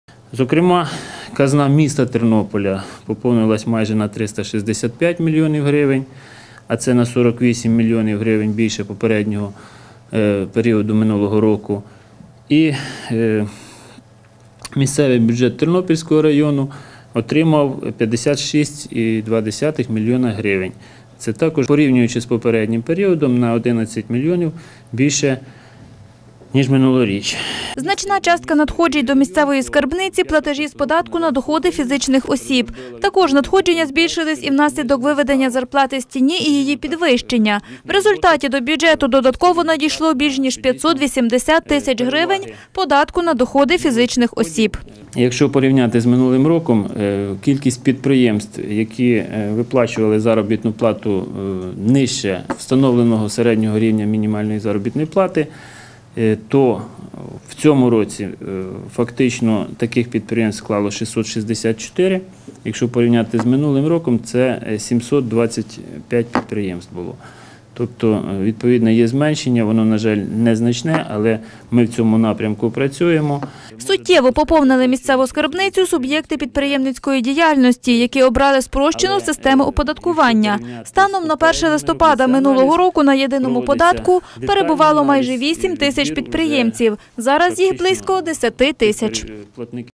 За 10 місяців у податкової зросли доходи
За 10 місяців цього року надходження в Тернопільську об'єднану Державну податкову інспекцію зросли. За цей час до зведеного бюджету від платників податків Тернополя і Тернопільського району надійшло майже 900 мільйонів гривень платежів. Це на 203 мільйони гривень більше, ніж за аналогічний період минулого року. Як вдалось досягти таких результатів розповів на прес-конференції начальник Тернопільської об'єднаної ДПІ Петро Якимчук.